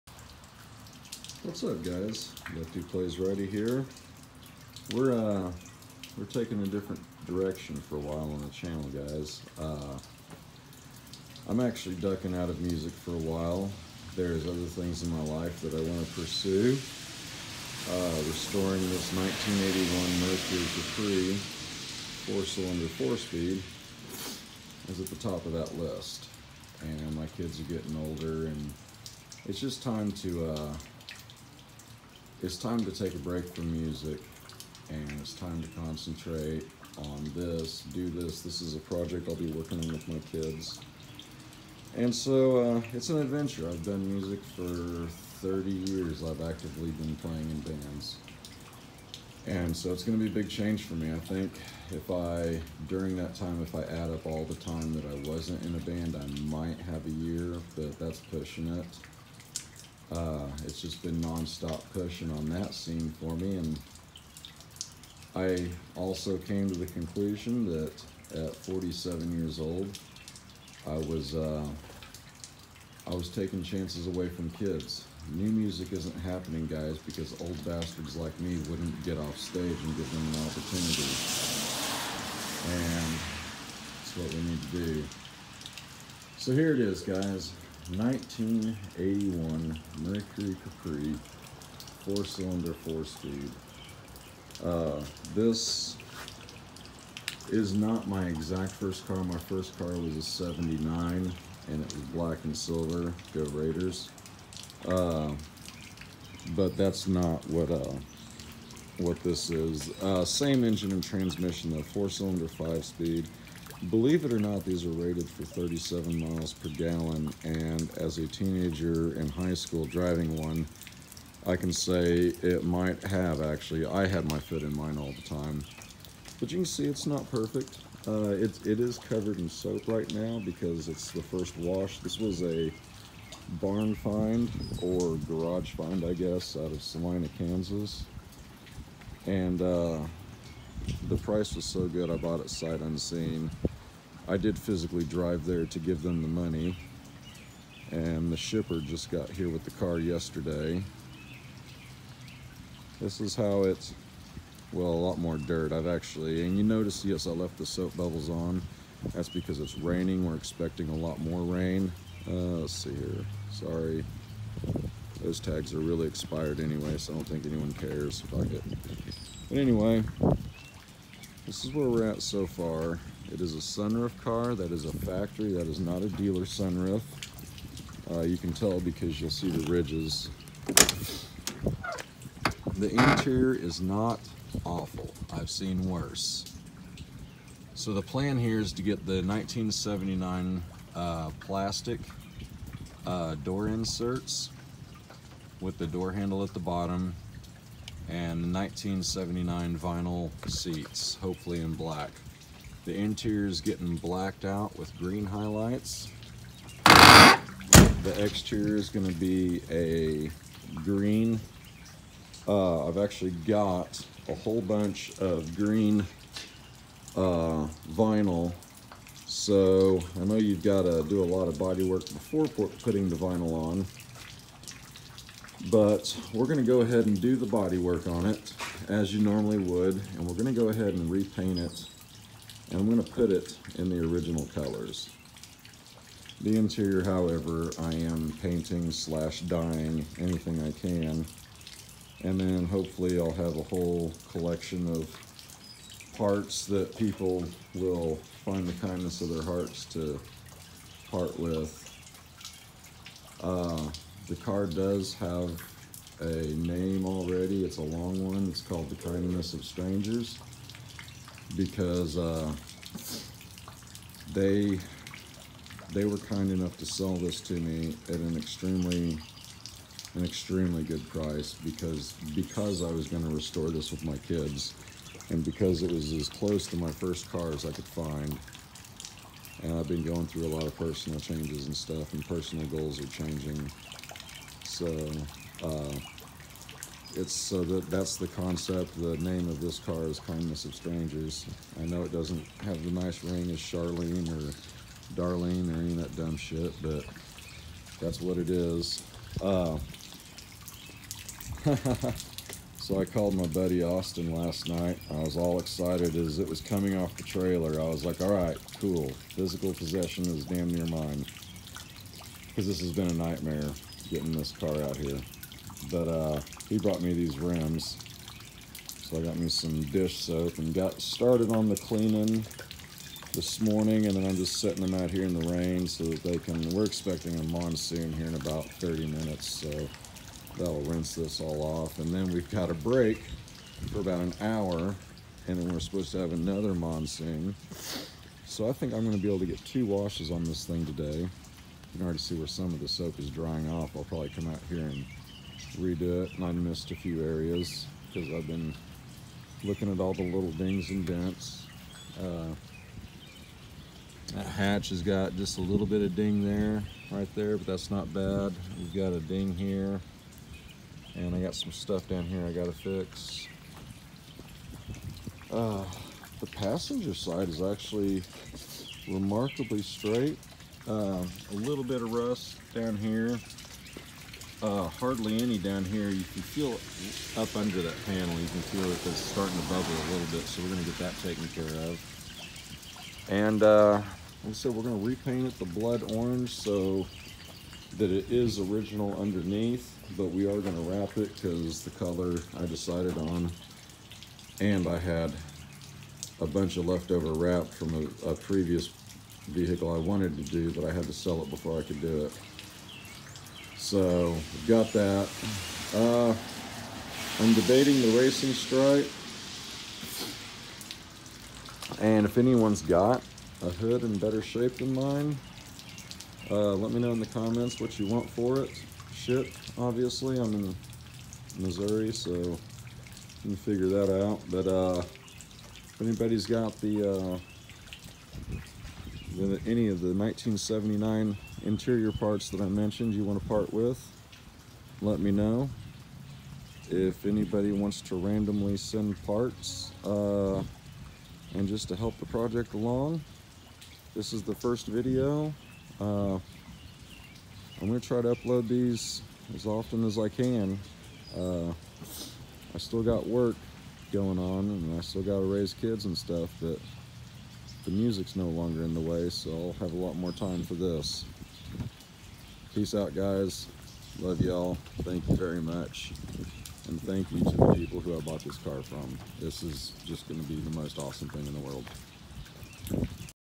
1981 Mercury Capri restore #1
This is my midlife crisis rebuild. It’s as close to my first car as i could find, and I’m gonna roll with it!